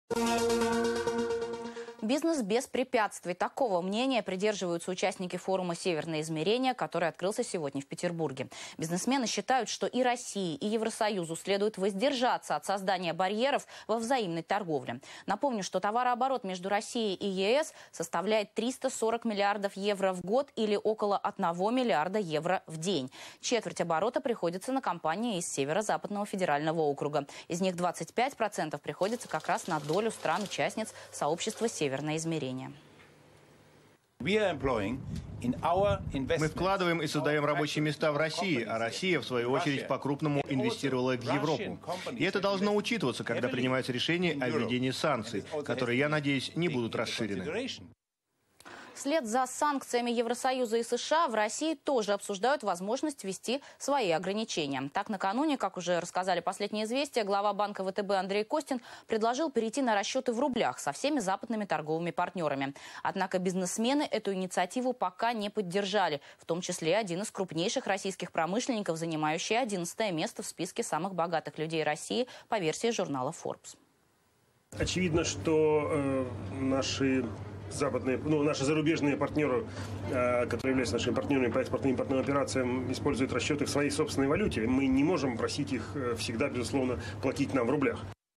Бизнесмены выступили против торговых барьеров между Россией и Евросоюзом
Воздерживаться от создания барьеров для взаимной торговли призвали политиков участники форума «Северное измерение». Форум открылся в Петербурге в четверг. Предприниматели надеются, что расширения взаимных санкций не будет.